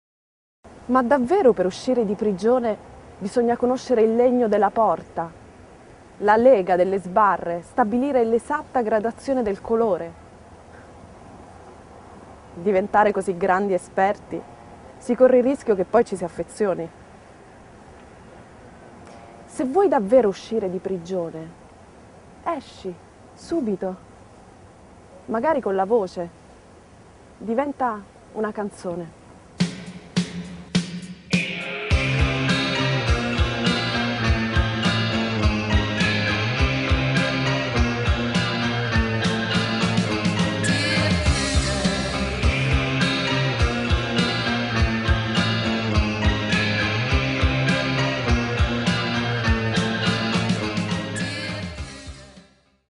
Se Non Ora Quando_Isabella Ragonese
Lo spot di Francesca Comencini con Isabella Ragonese sulla giornata di mobilitazione del 13 febbraio 2011